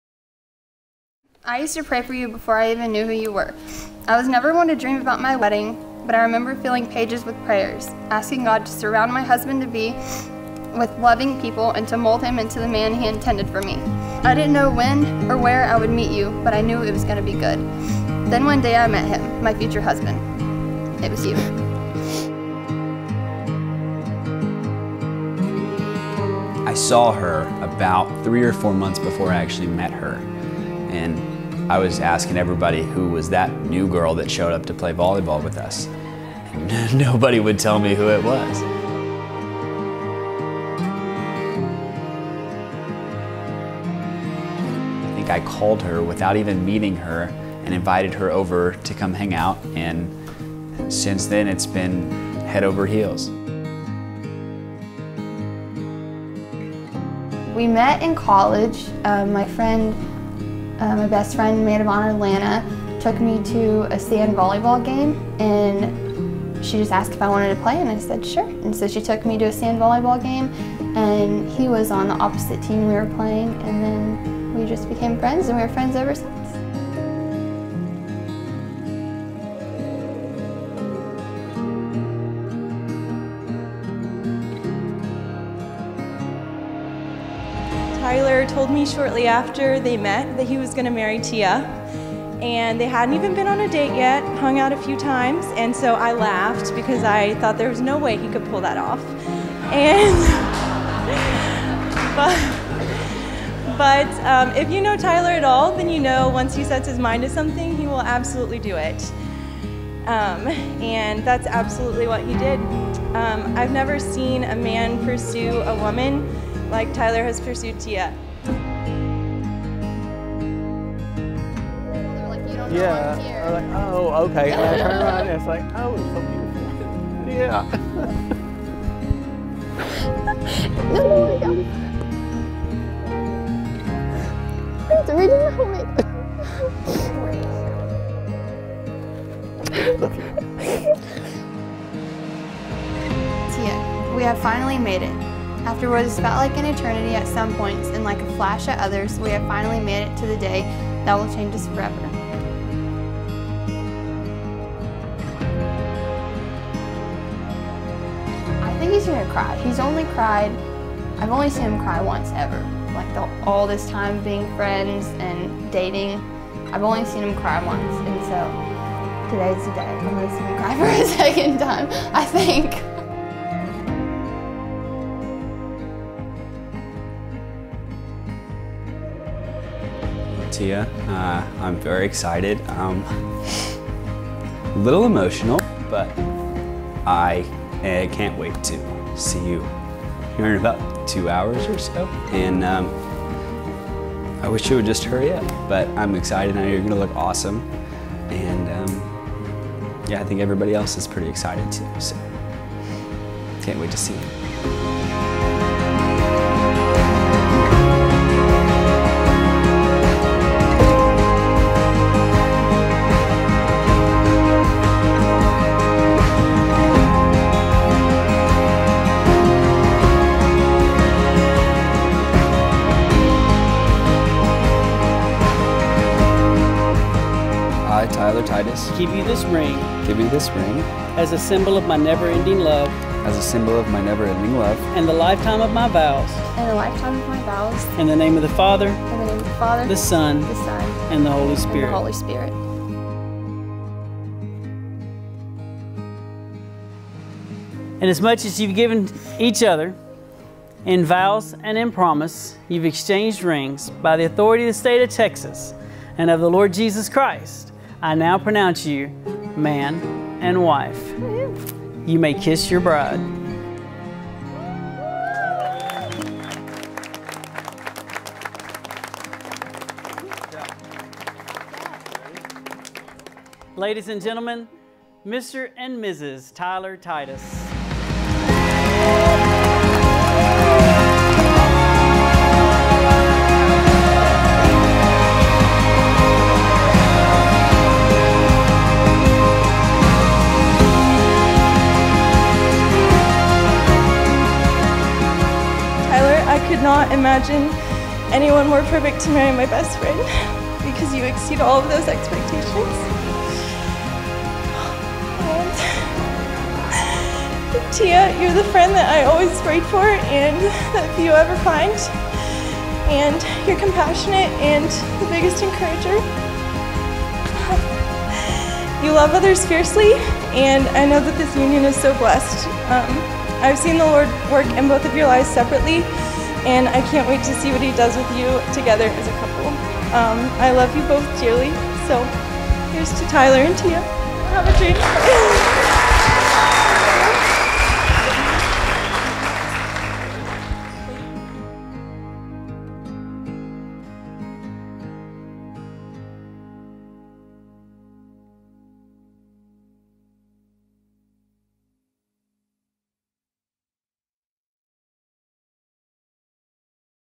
Tia + Tyler | Trickle Creek Events in Weatherford, TX Wedding Film
We had such an amazing time filming and photographing Tia and Tyler's incredible wedding at Trickle Creek Events in Weatherford, TX.